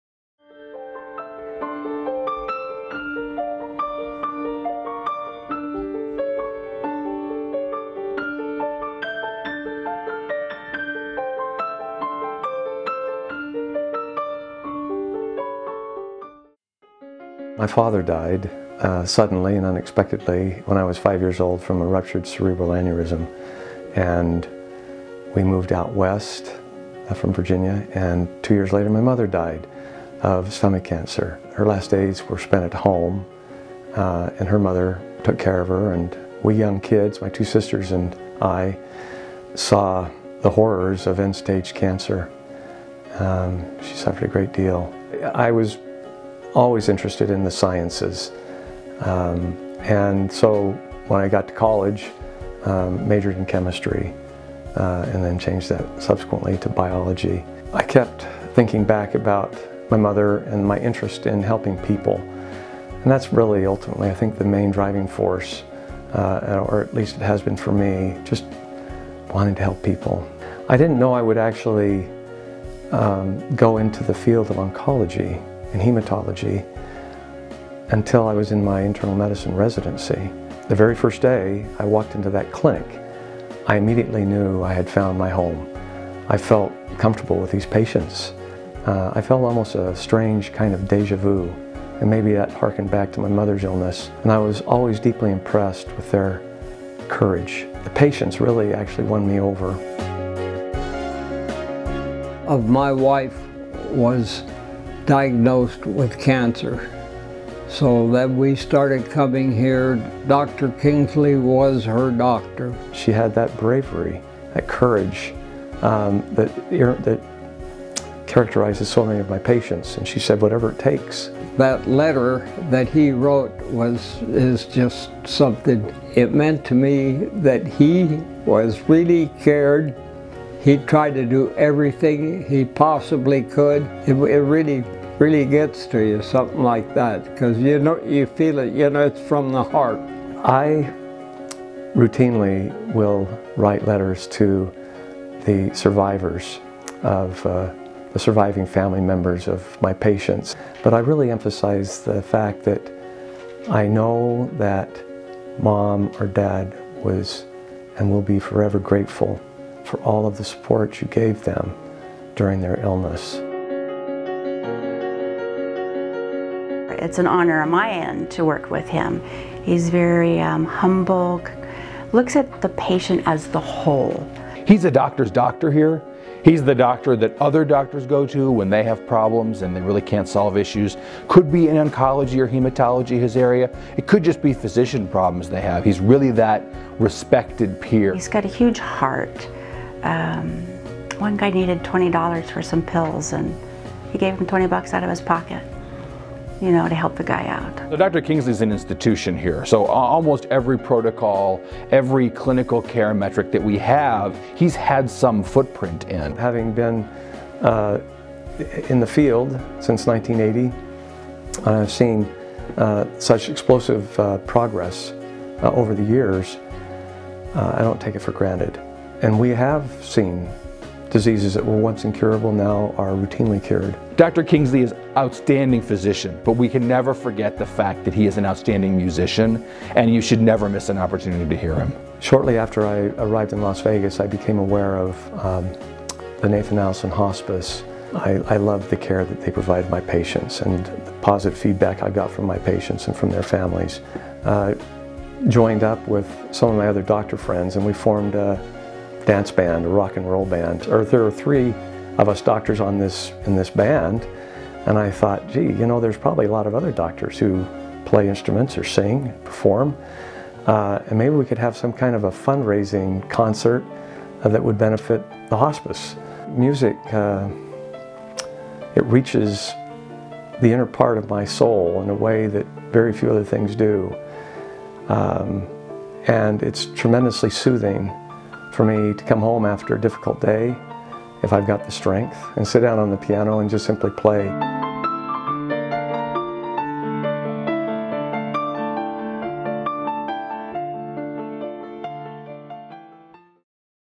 Edwin Kingsley, MD | 2016 Inspired Excellence in Healthcare Award Recipient
In this video, we sit down with the 2016 Inspired Excellence in Healthcare Award Recipient, Edward Kingsley, MD.

Dr. Kingsley is a medical oncologist-hematologist and one of the founding members of Comprehensive Cancer Centers of Nevada (CCCN), where he has treated patients with cancer and serious blood disorders for more than 27 years. He has authored numerous publications and continues to be actively involved in clinical research at CCCN.

Thank you, Edwin Kingsley!